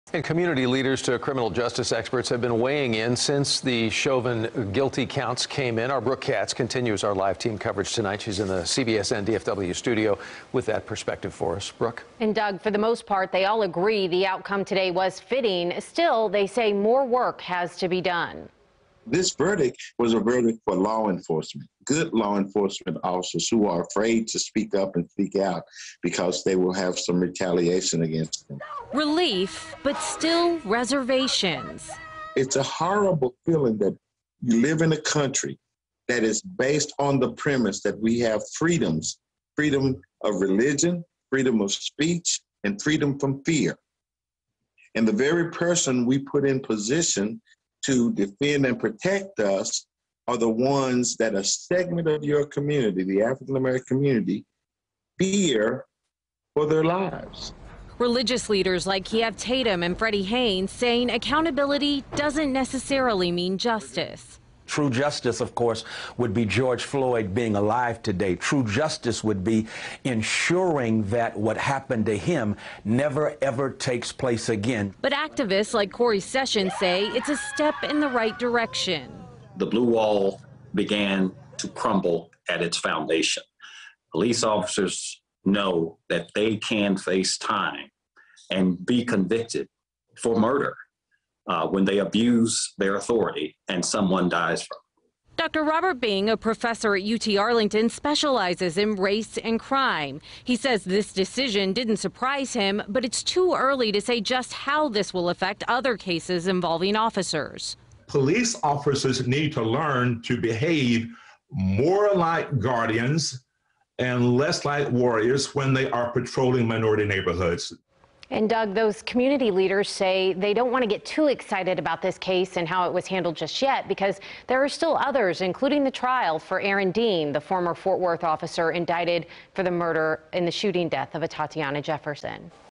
Dallas Community Leaders, Criminal Justice Experts Weigh In On Chauvin Guilty Verdict
They said the outcome was fitting, but more work needs to be done.